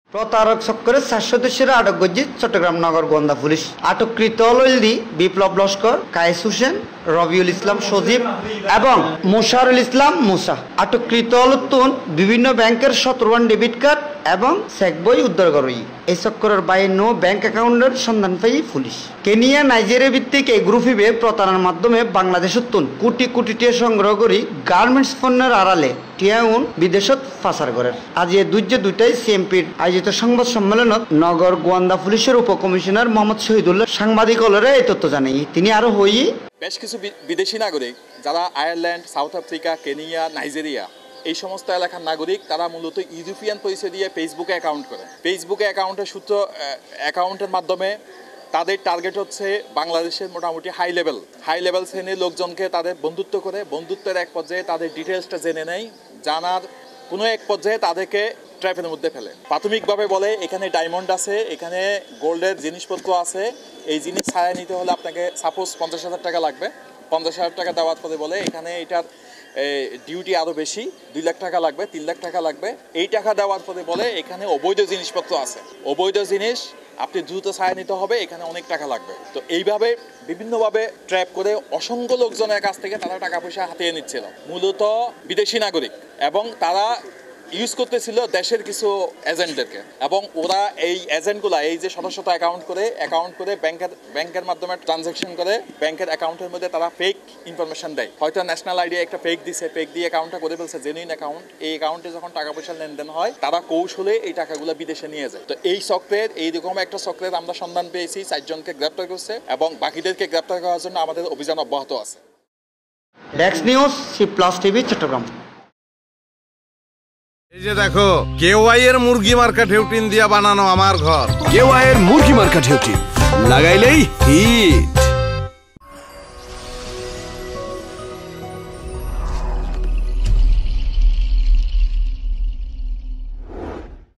চট্টগ্রাম নগর গোয়েন্দা পুলিশের অভিযানে বিভিন্ন জায়গা হতে প্রতারক চক্রের চার সদস্যকে আটক
চট্টগ্রাম নগর গোয়েন্দা পুলিশের অভিযানে বিভিন্ন জায়গা হতে প্রতারক চক্রের চার সদস্যকে আটক করা হয়েছে। আটকৃতদের কাছ থেকে বিভিন্ন ব্যাংকের ১৭ টি ডেবিট কার্ড এবং চেকবই উদ্ধার করা হয়।